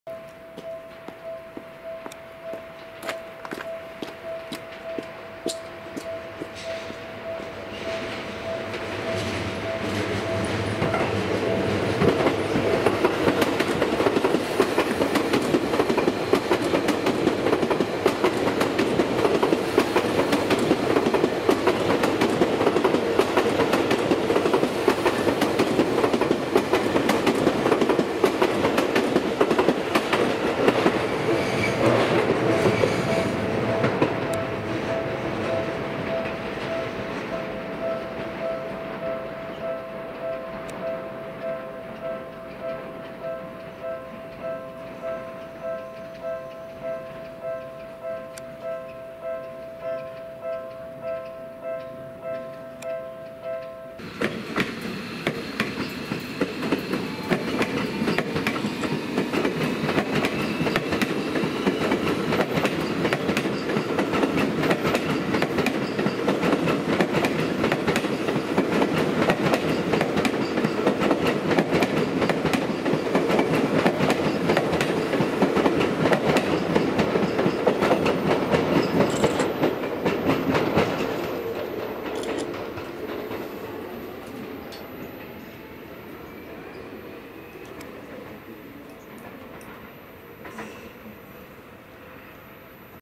「映画ドラえもんのび太の新恐竜」ラッピング電車（西武40000系）
新しく西武鉄道に入線したばかりの最新鋭40000系電車のロングシート車が、さっそくラッピング電車に早変わりしています。2020年1月11日から、「映画ドラえもんのび太の新恐竜」ラッピング電車として運行を開始です。今回は走行シーンを所沢付近と小手指駅で撮影しました。